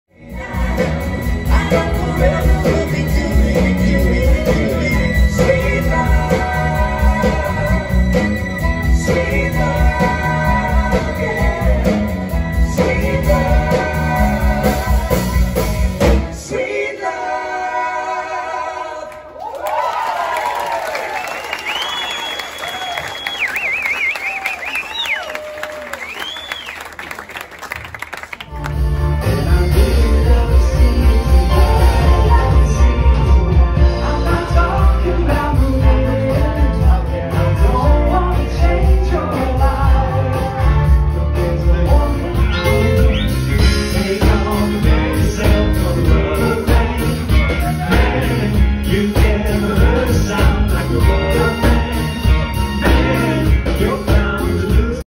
Reminiscing Live
Phone videos from our debut at the Charthouse Live in Lakeville, MN